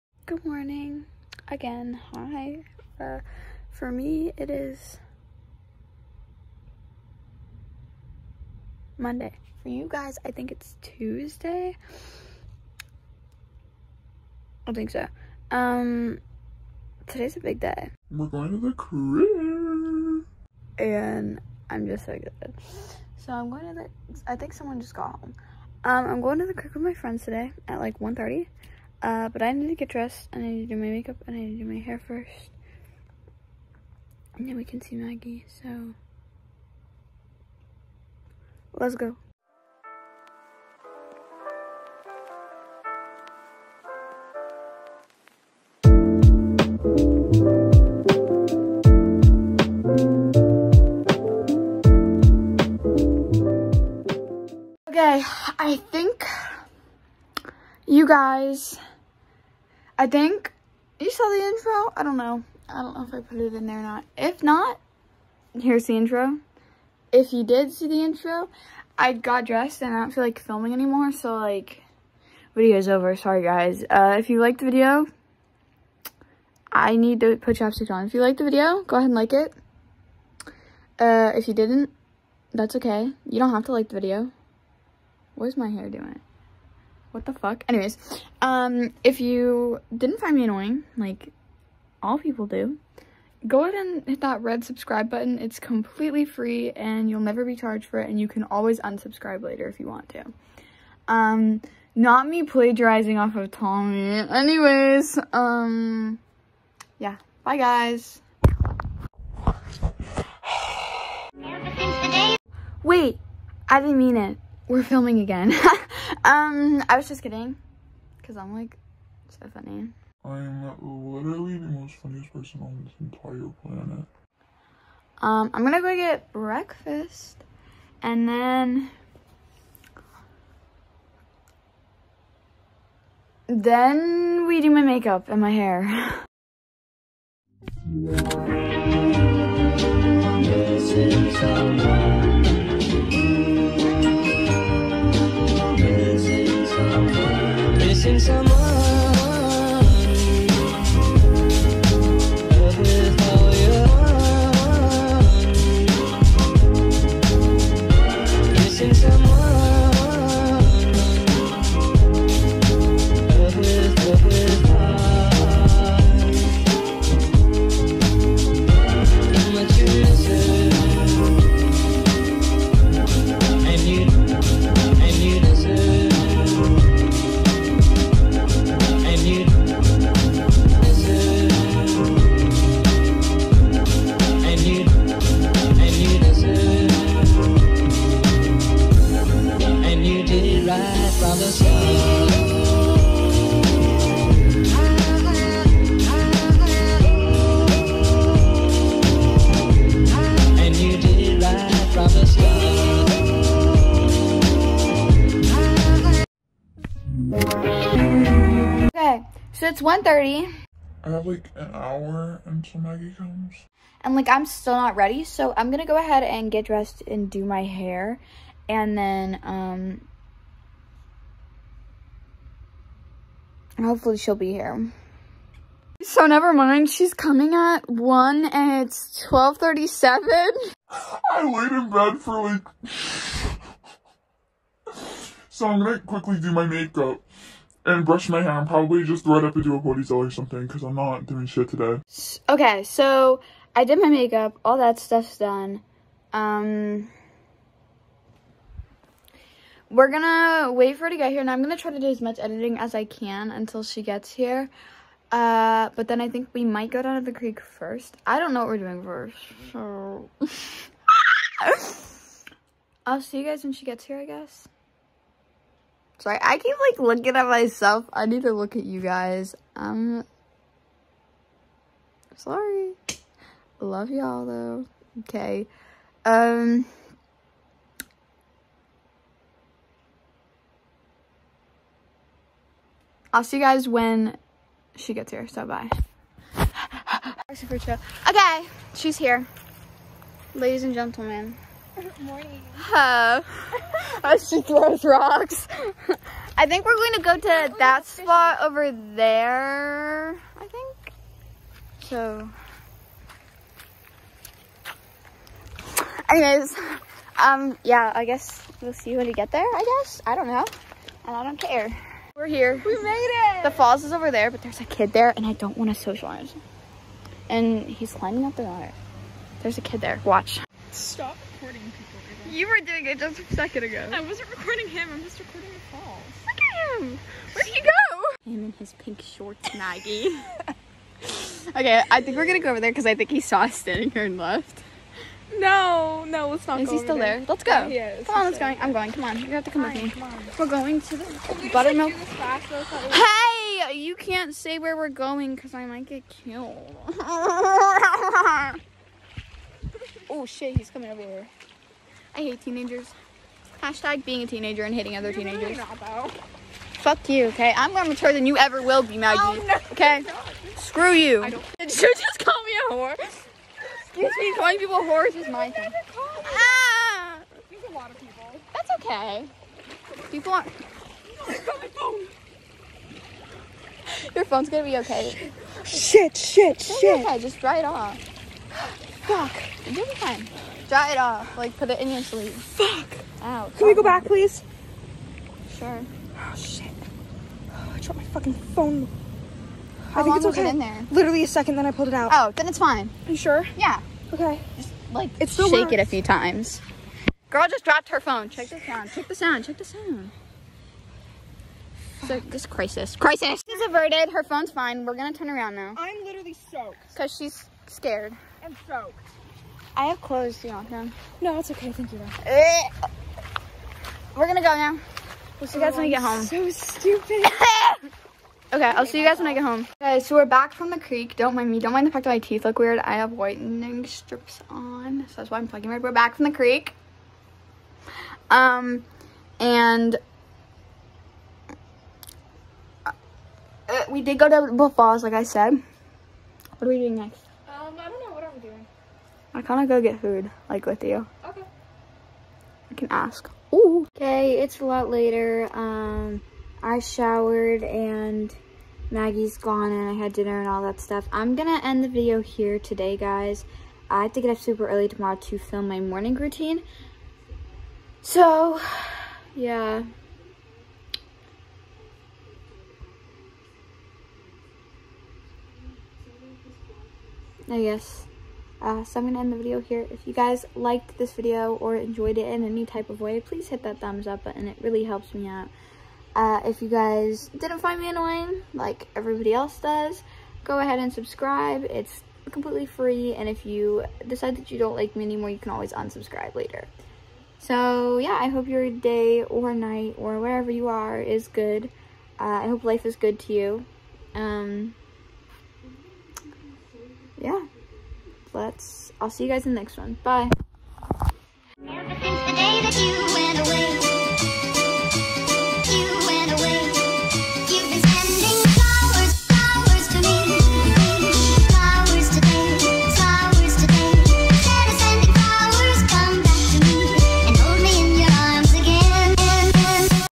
Vlog Day #3 | Creek| 8/6/21
today we ended up going to the creek and spending. time w my friend if this is somthing u would like to see more often go ahead and tell me, or make a suggestion on what i should do next